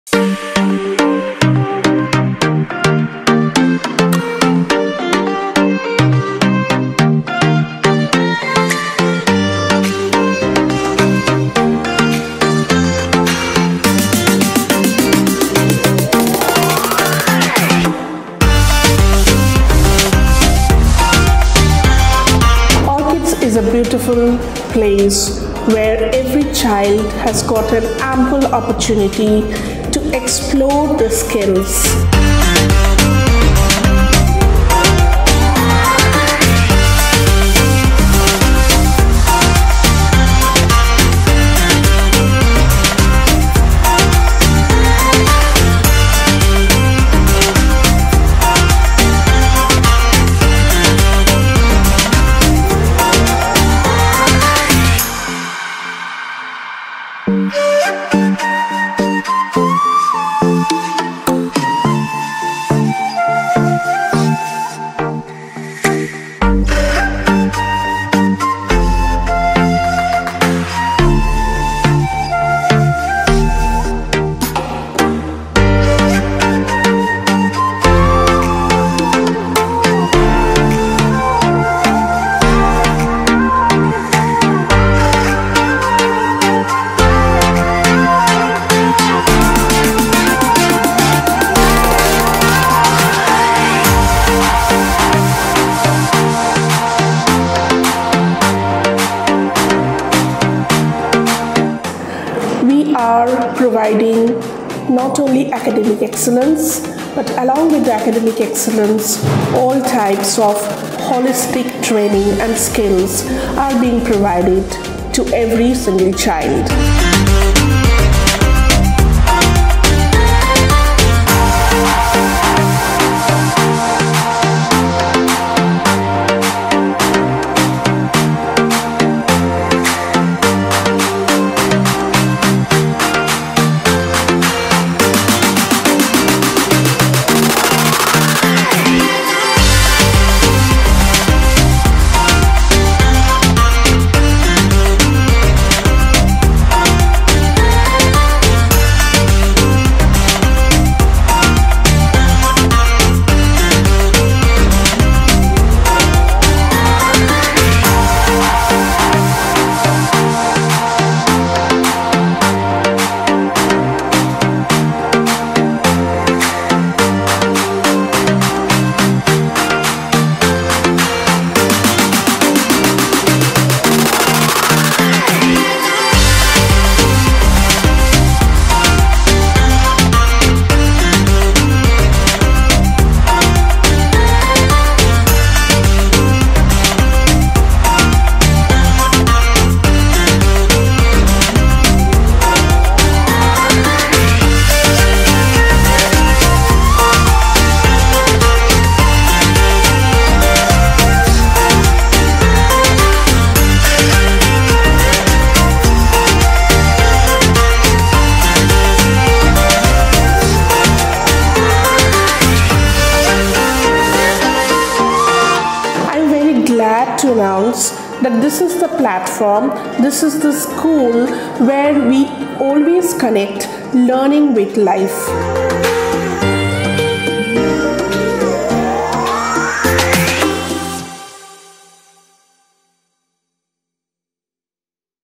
Walk Through | ORCHIDS The International School, Kolkata
Infrastructure indeed plays an important role in the smooth functioning of school’s administration. The rarely seen is the role of infrastructure in building the confidence and attitude in a child. The glaring infrastructure of a school can elevate the confidence level in a child.
Walk the virtual tour through the infrastructure of our school, New Town Kolkata. We go extra mile to make them shine for our children.